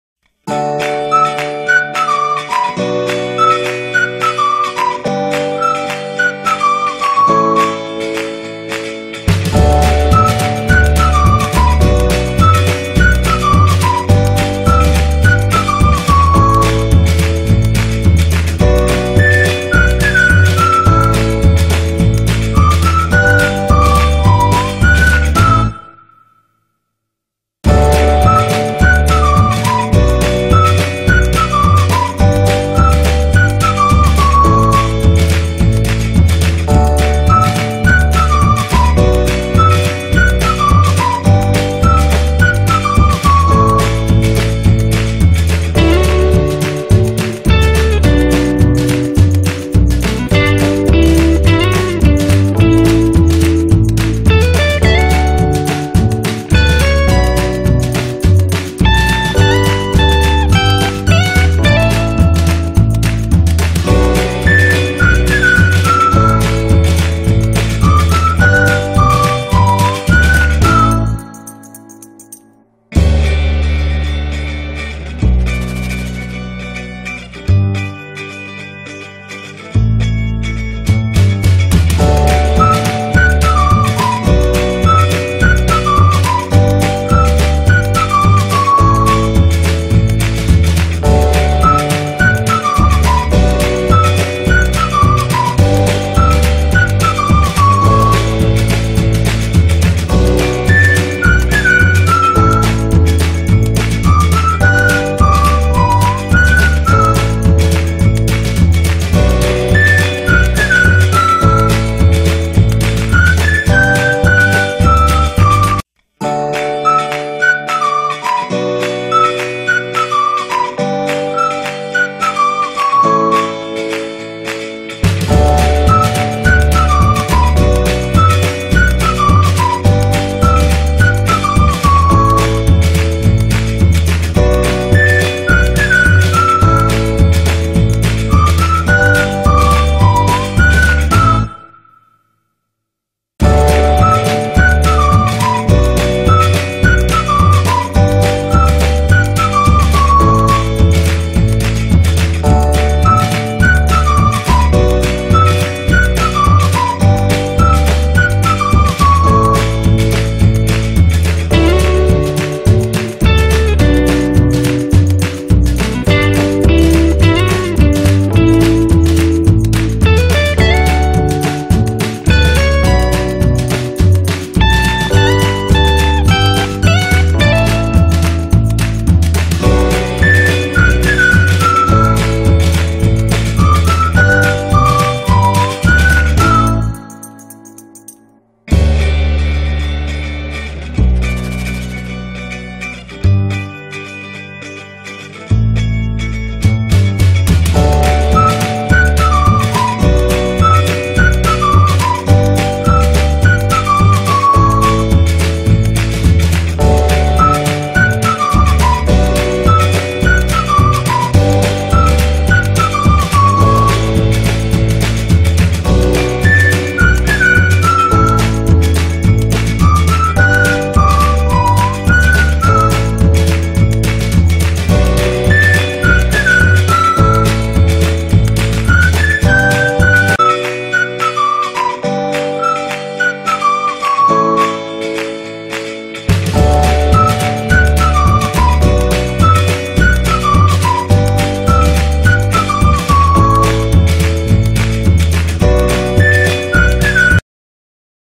ISKF Barbados - Journey to The ISKF World Karate Competition in South Africa 2016
The ISKF Barbados team traveled half the globe to reach the ISKF Karate Championships in South Africa 2016 - watch their journey.